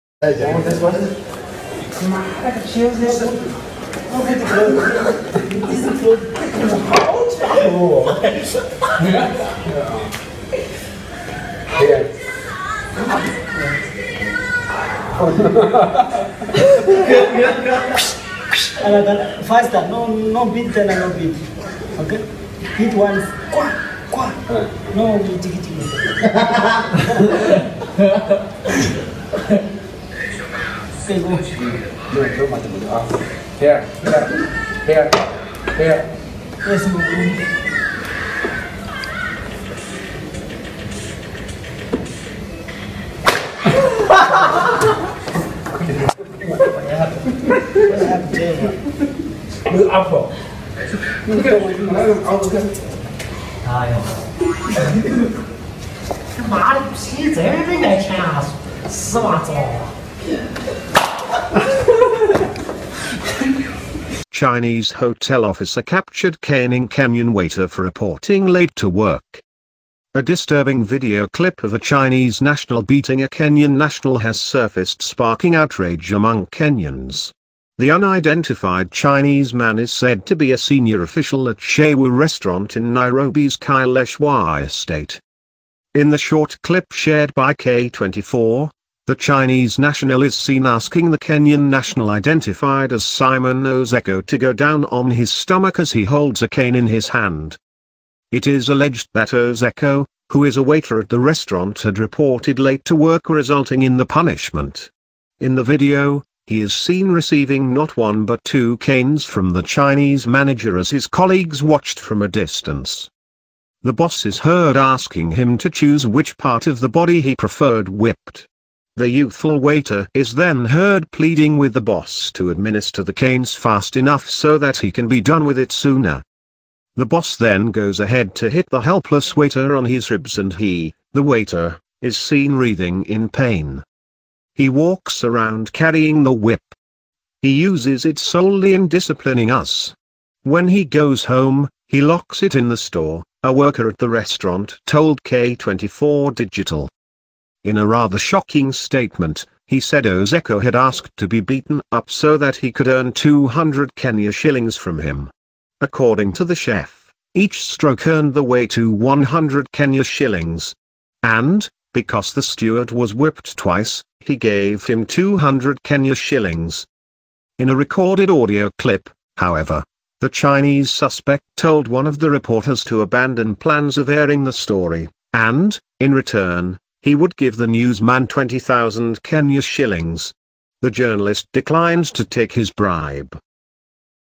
Shocking as Chinese boss whip Kenyan waiter at hotel,is it back to slavery?
A video recording showing a senior Chinese hotel worker whipping a Kenyan male waiter for allegedly reporting late to work has emerged.
Police in Kenya arrested four Chinese nationals, local media reported on Sunday, after a shocking video began circulating of one of them publicly flogging one of his employees, a Kenyan national, for allegedly being late to work.
In the video  the Chinese man believed to be the head chef at Chez Wou Restaurant in Kileleshwa, Nairobi is seen punishing the seemingly youthful waiter who is alleged to have arrived late for work
The chinese employer came up with defense that it was on willingness basis, Kenyans reacted on the viral video seeking the government intervention and arraign the chinese nationals for judgement

What I see from this video clip is that, the assaulted person is even telling the Chinese guy to cane him fast. It is not a game because after the African man is seen rubbing his back and entering an invisible room while the Chinese guy is heard laughing. My view is that, even if the man is seen lying down flat on the ground to be canned by his employer, there is a high likelihood the employer wanted to humiliate the man by beating him like a slave or a prisoner. The fact that the man has no medical report to show how he was injured is irrelevant. My honest opinion is that the government must take disciplinary action to foreigners who disrespect Kenyan citizens and if possible their visa's cancelled and deport such foreigners back to their country,David Gichuhi reacted